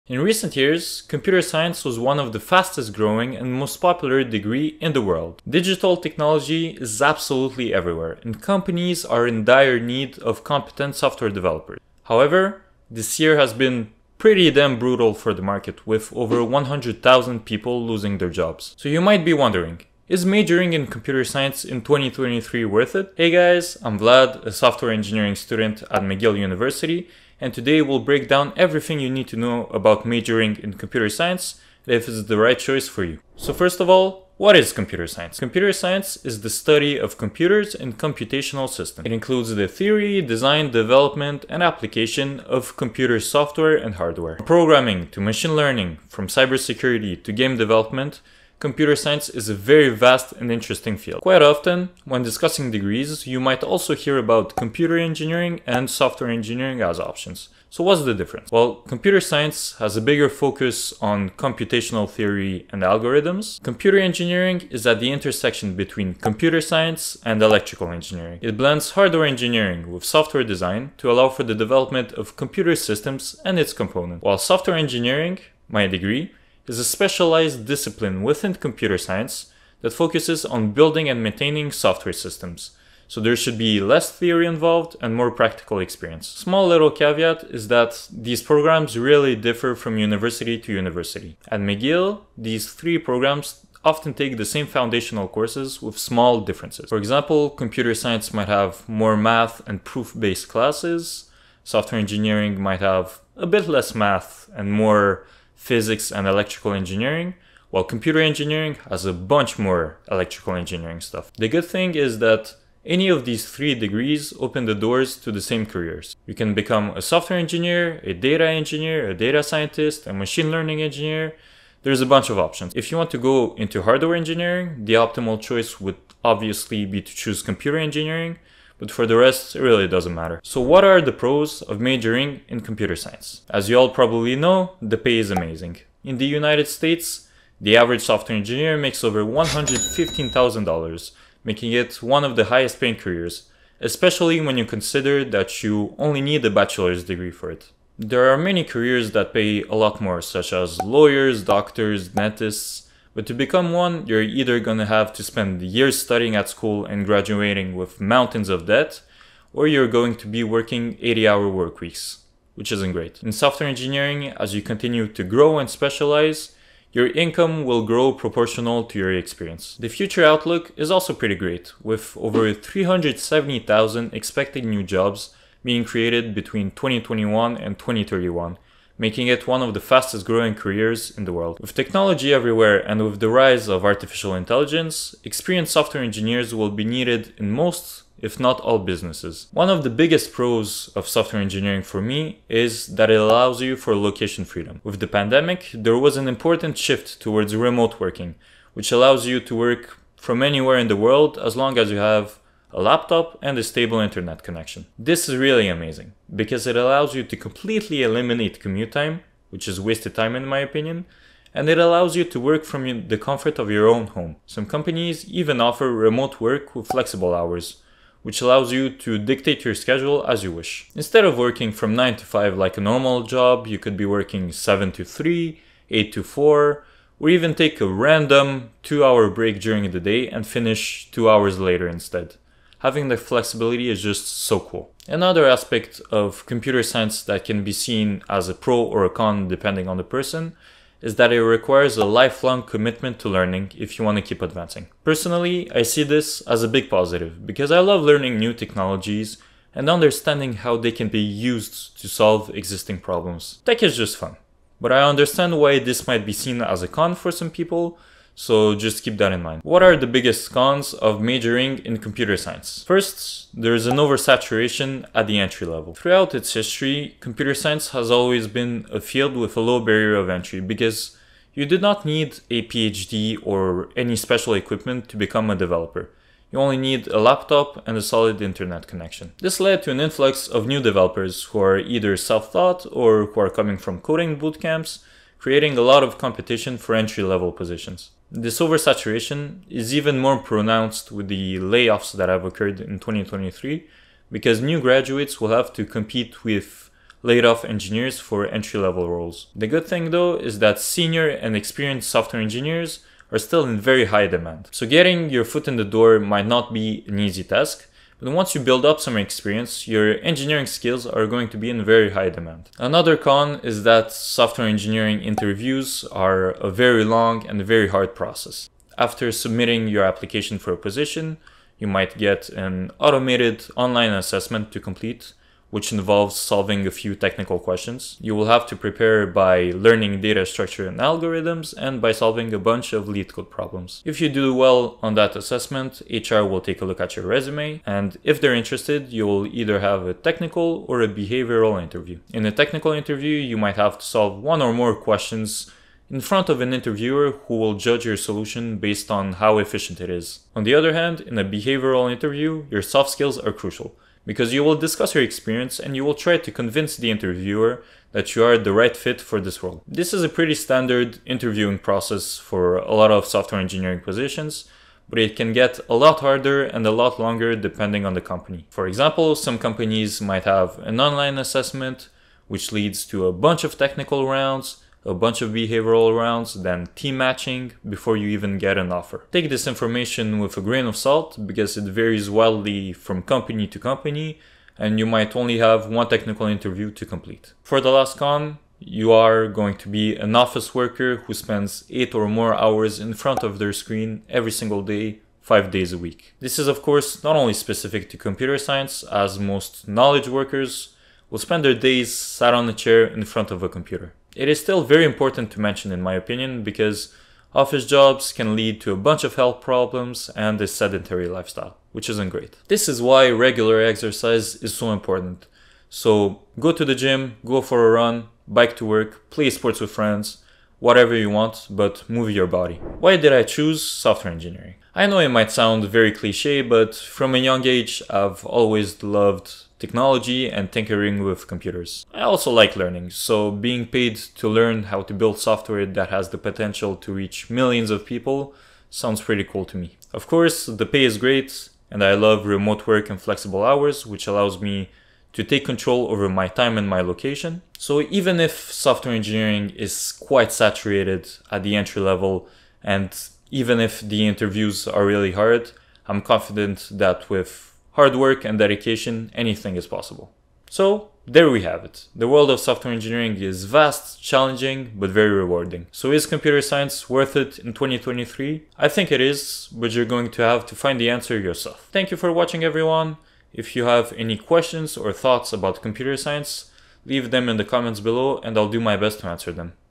Is Computer Science Worth It in 2024 ?
This video is about a highly requested subject:
Is majoring in computer science a good idea in 2024 ?

We will discuss the market conditions, the pros, the cons and my personal opinion. In the end, the choice is yours !